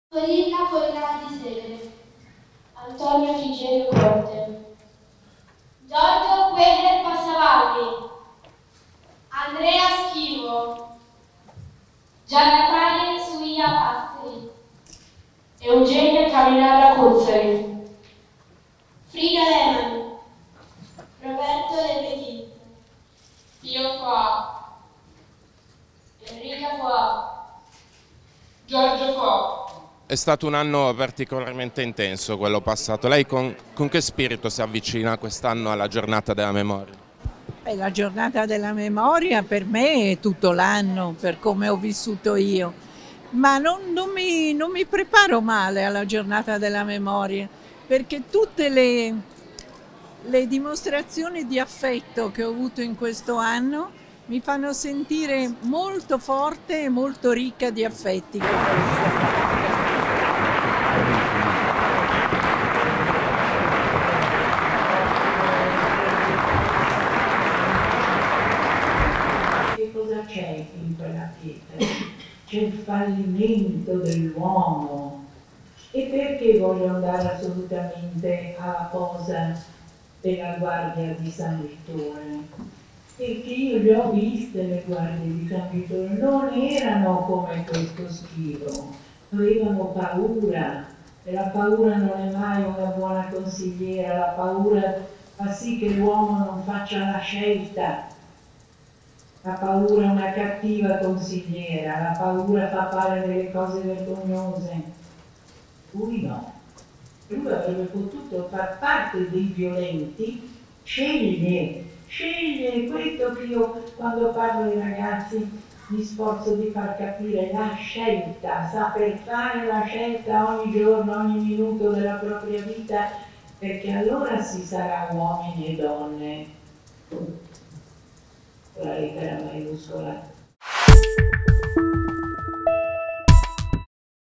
Milano, Liliana Segre sopravvissuta Auschwitz incontra i bambini | Ecco le nuove pietre d'inciampo
In Consiglio Comunale Liliana Segre, sopravvissuta di Auschwitz, racconta il dolore della deportazione e l'importanza di "fare la scelta giusta": "Non cedete all'odio".